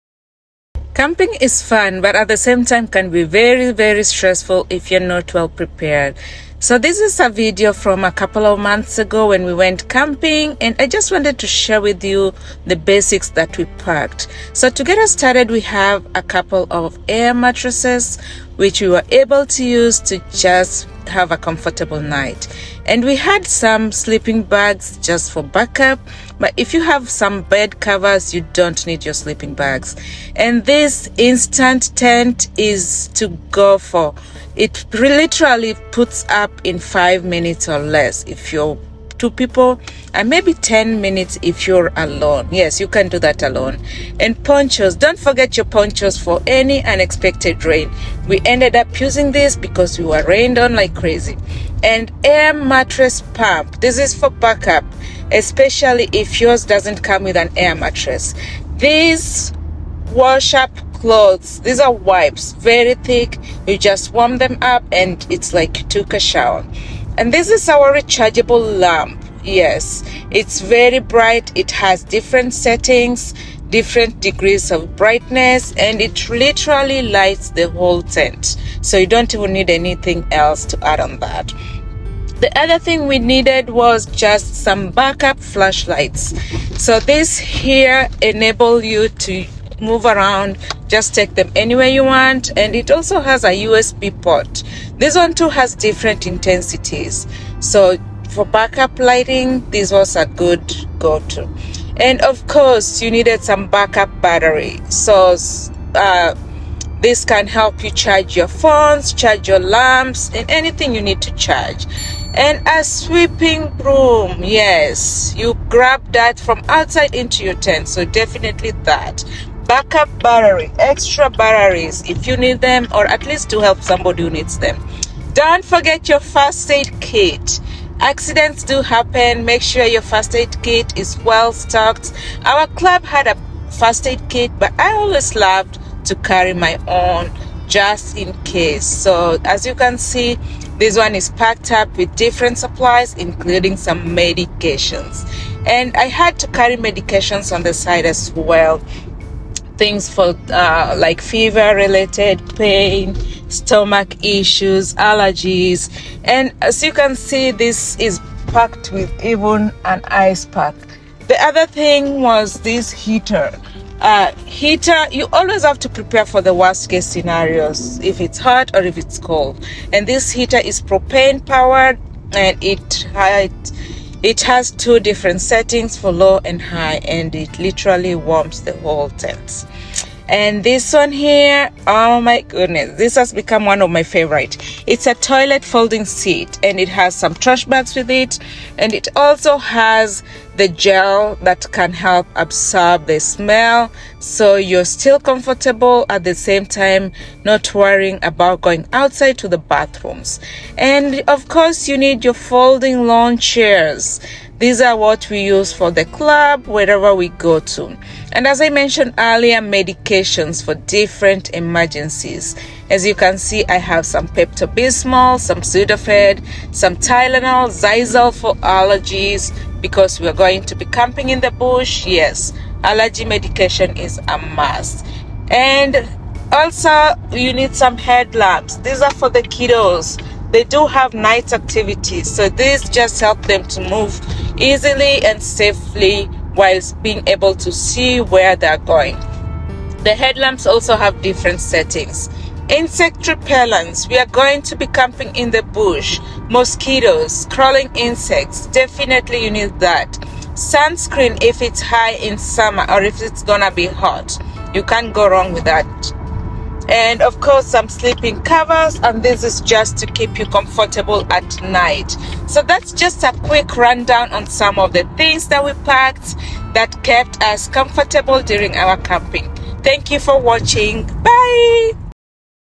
What You Need To Pack For Camping |Camping Essentials
Camping can be fun and stressful at the same time. Sometimes all you need is to pack these essentials for a comfortable camping experience.